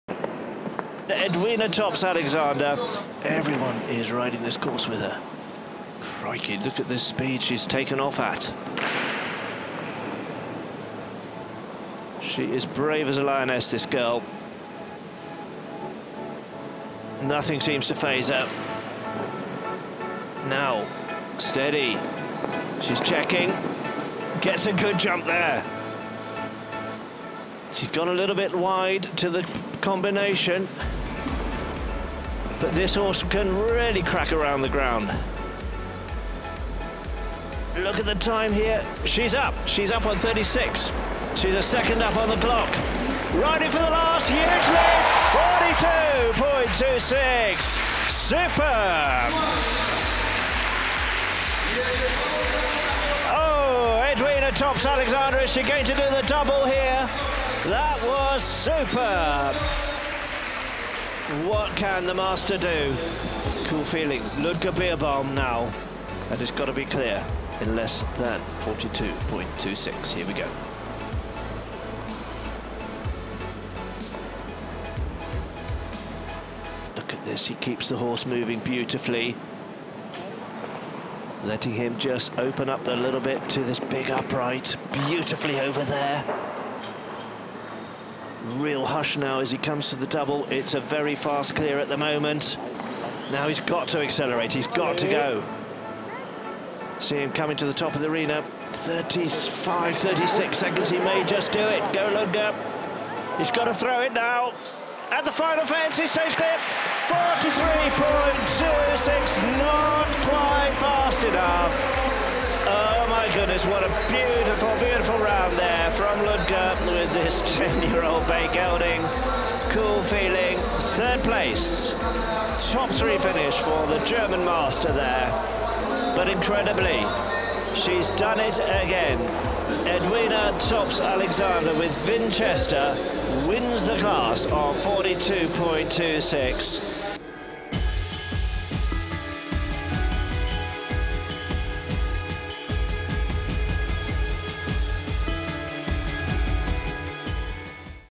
Edwina Tops-Alexander ahead of Ludger Beerbaum in La Coruña | Longines FEI Jumping World Cup™
Edwina Tops-Alexander did it again. After already winning the previous edition of the Longines FEI Jumping World Cup™ in La Coruña, she showed a superb round on her 9-year old Vincester. Even Ludger Beerbaum had no chance against the Australian Jumper and finished third. The 2nd place went to Emanuele Gaudiano from Italy.